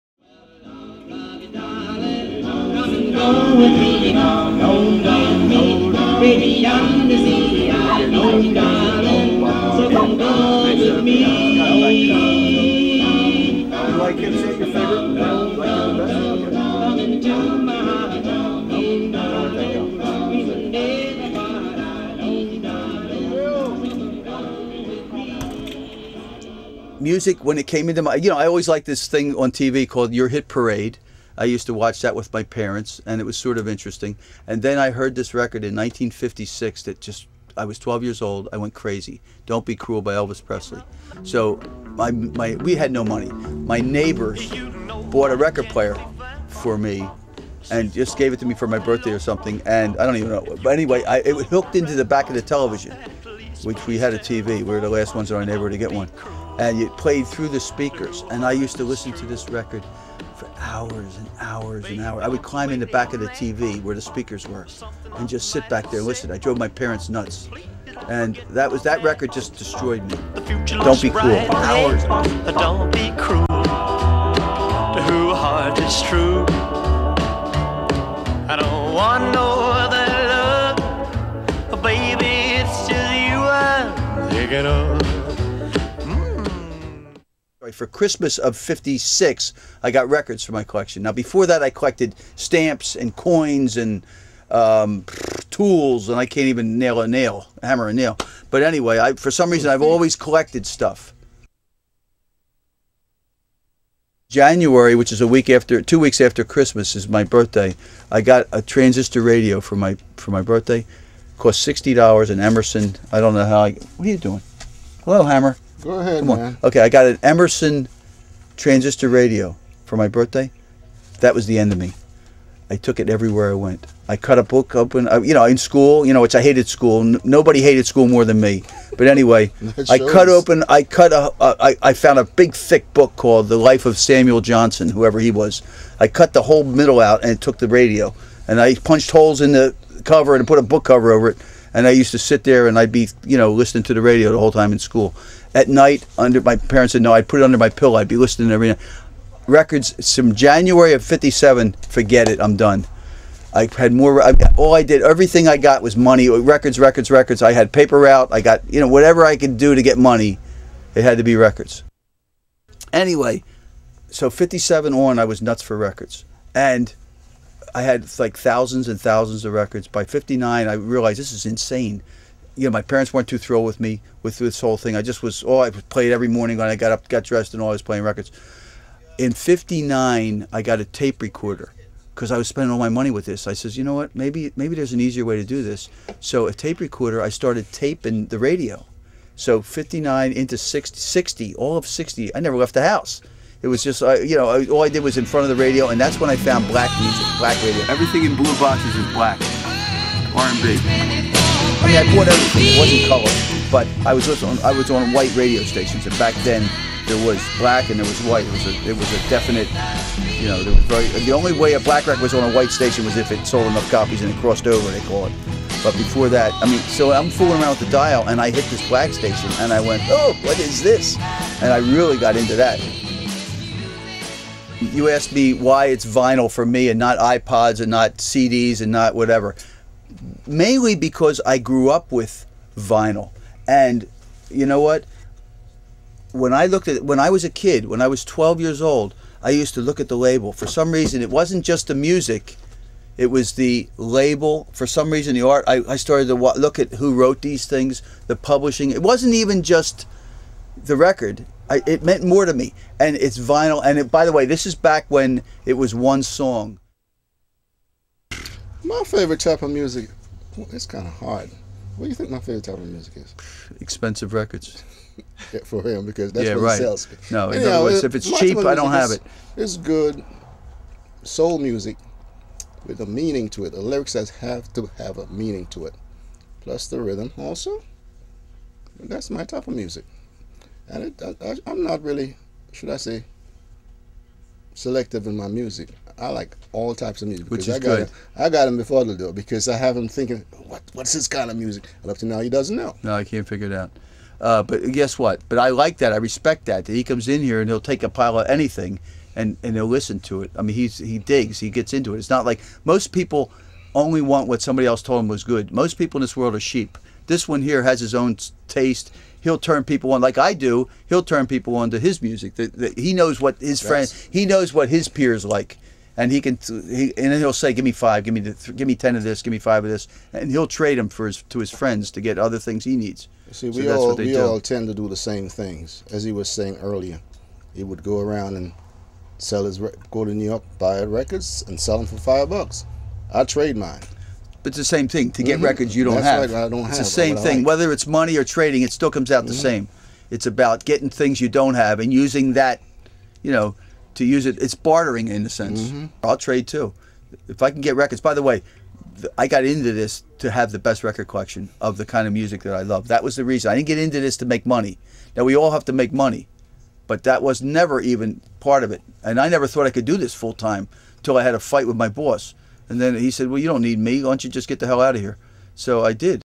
Good Housekeeping Video featuring Michael Neysmith and Val Shively
Michael Neysmith makes a visit to Val Shively's R&B Records store in Upper Darby, Pennsylvania